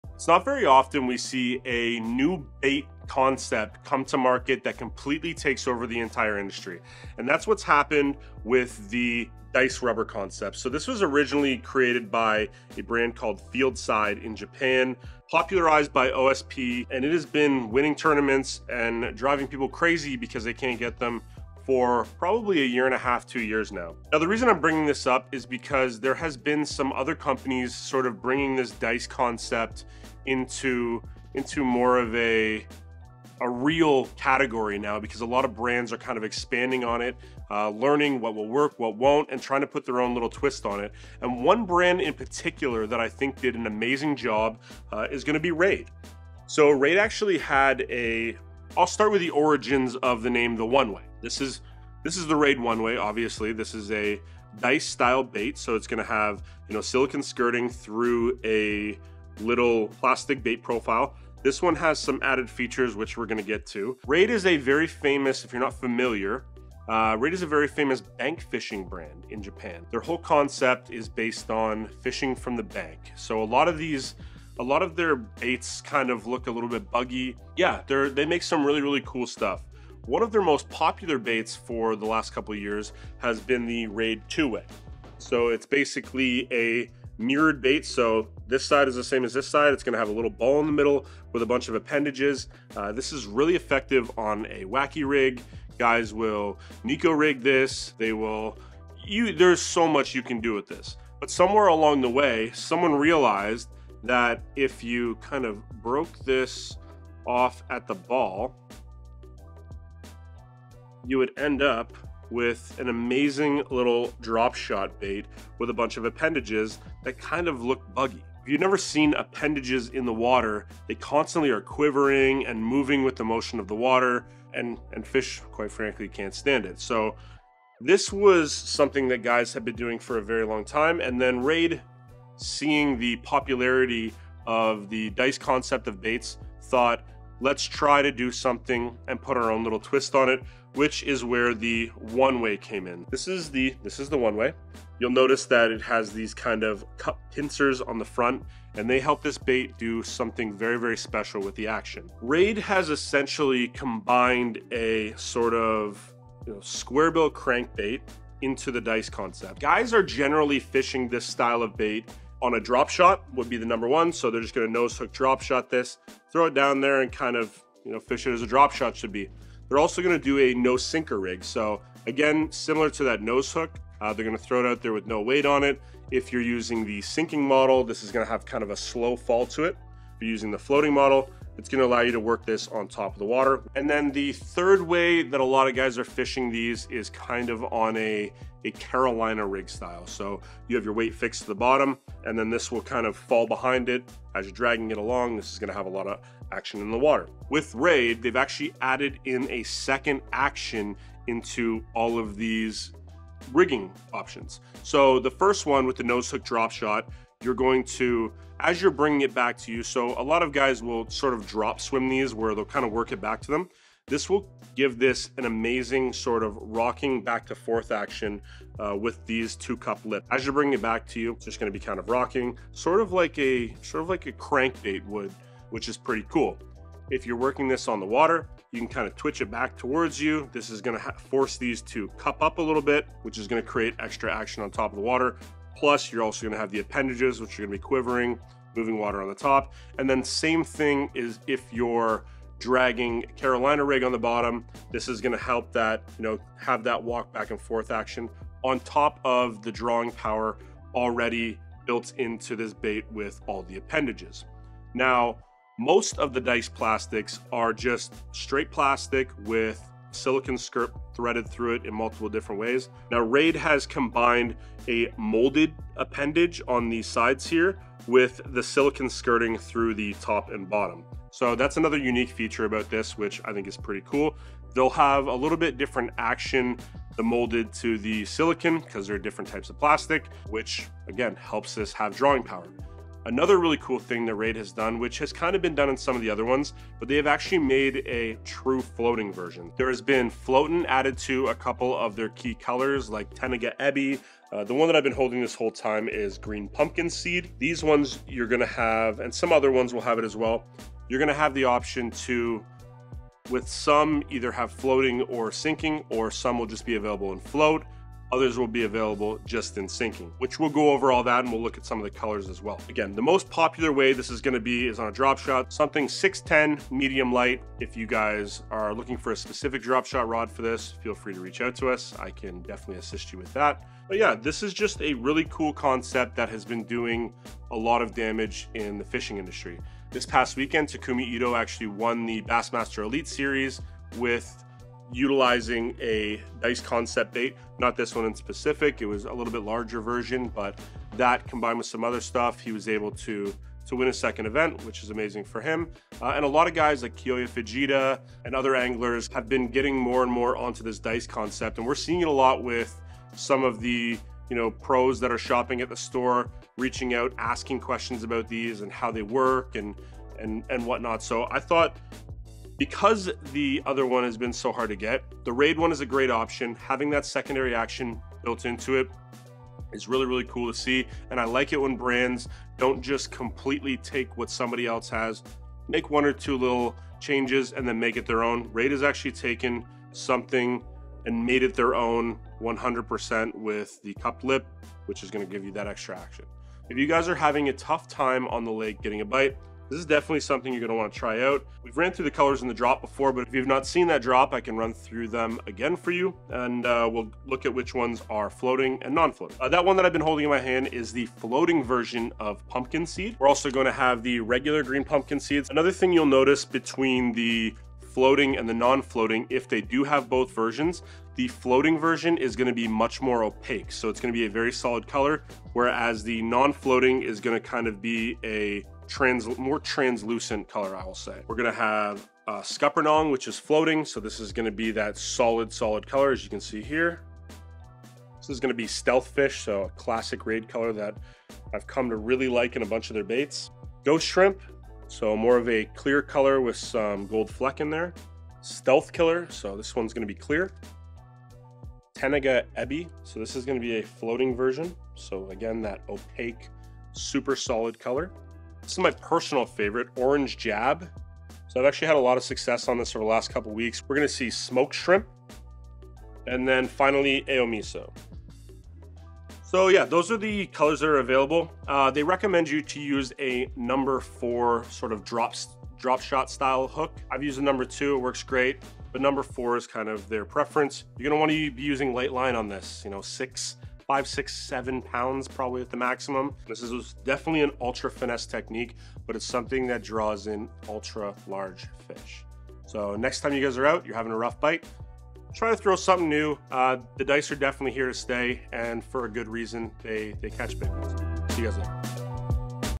Dice Bait: The Future of Fishing?
One of the most popular and fastest new growing bait categories in the industry - the "Fuzzy Dice". Created by Field Side Japan and popularized by OSP,   "fuzzy dice"  is appearing all over the industry. Raid Japan put their own twist on their version of the fuzzy dice by adding poured wings along with traditional skirt material. It can be fished drop shot, no sinker rig, surface (floating models), free rig, Carolina rig, the options are endless!

0:54 - Raid Japan 1Way Hybrid

Hook Recommendations:
Hayabusa DSR132 Drop Shot Hook

Rod Recommendations:
G.Loomis NRX + Spinning Rod (821s NRR)